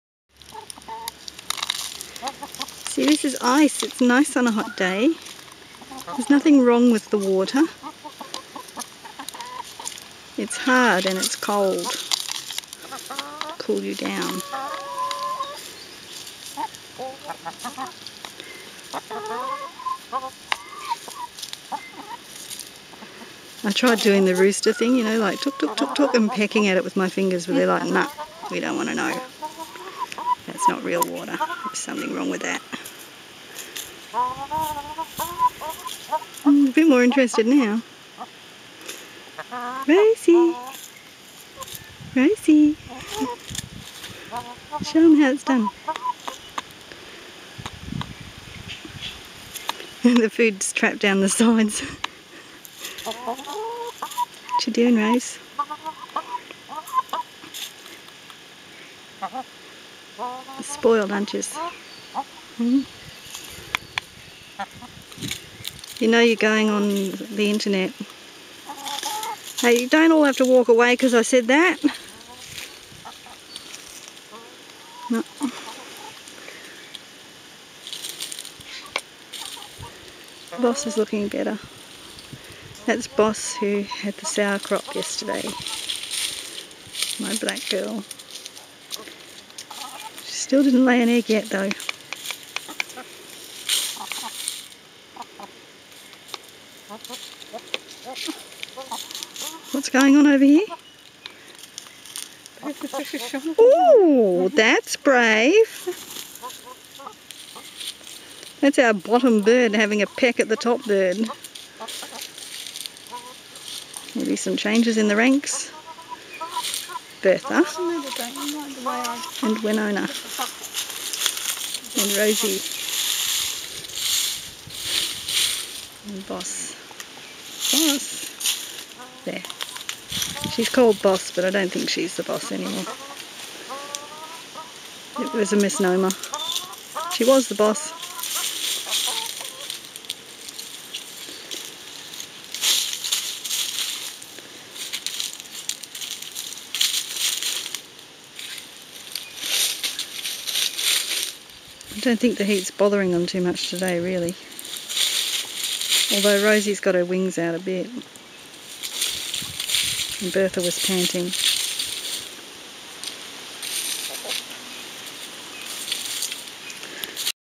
Chickens have ice for the first time
Introducing my chickens to ice to cool them down on a hot day.